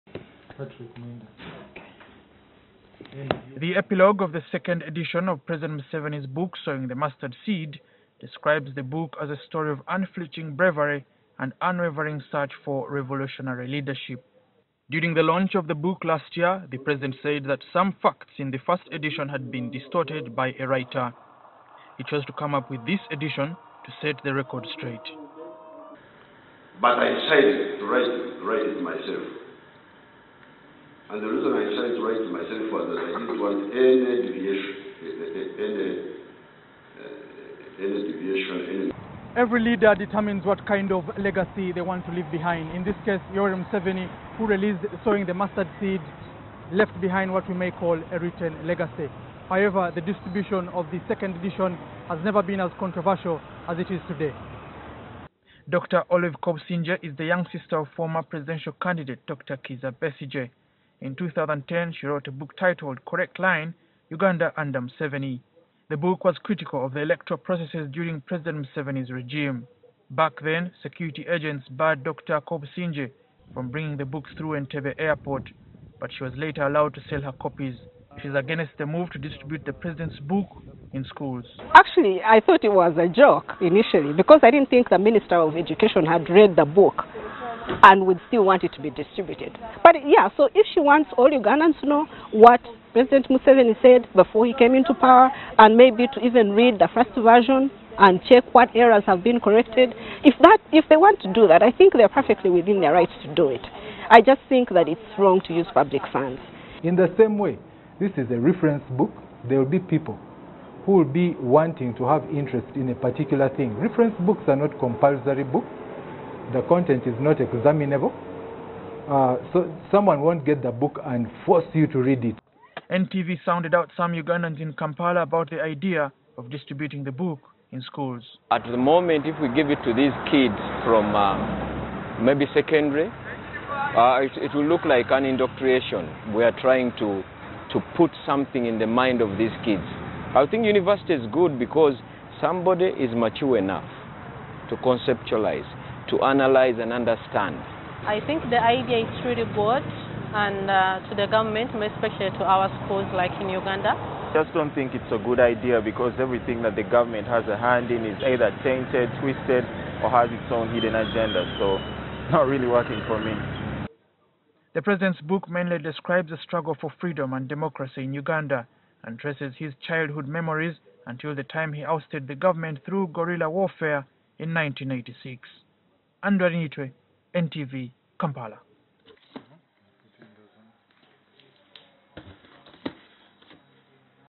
Author of "Correct line" denounces move to distribute president's book in school
The author of a book that's critical of President Museveni's leadership has denounced the recent move by the President to distribute 3,000 copies of his book "Sowing the Mustard Seed" to  public secondary schools. Dr. Olive Kobusingye, who's the younger sister of FDC leader Kizza Besigye says its wrong for the head of state to use public funds to distribute his book. In 2010, Kobusingye wrote a book titled the "Correct Line' which whipped up a storm.